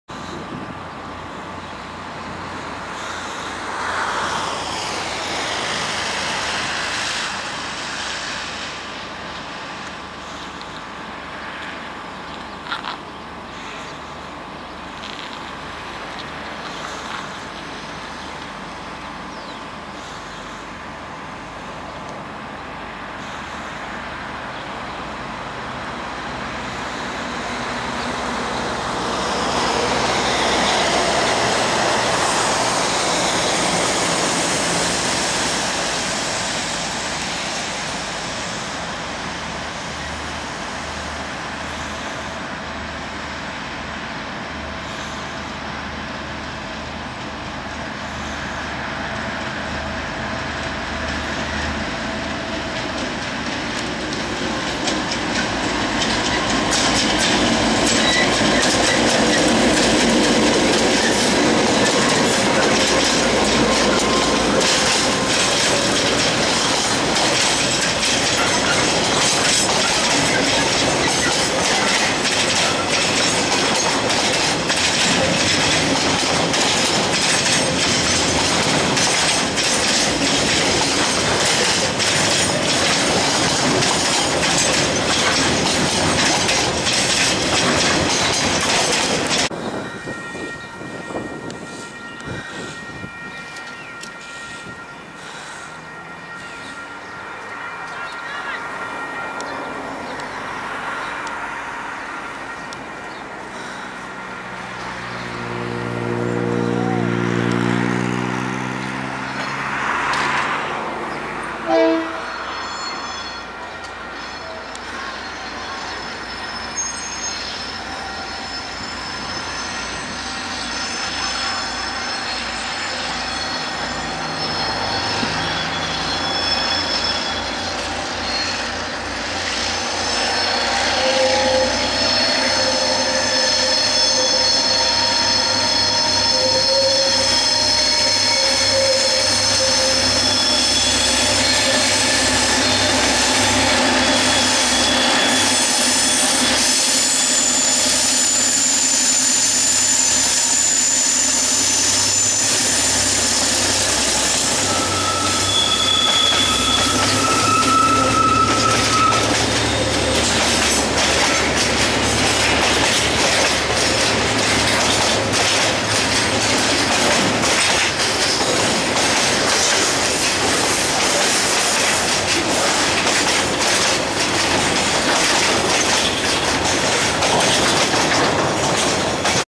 train 561 coming back from castlecliff 19,09,13
DC locomotive hauled milk powder train coming back from castlecliff, wanganui,new zealand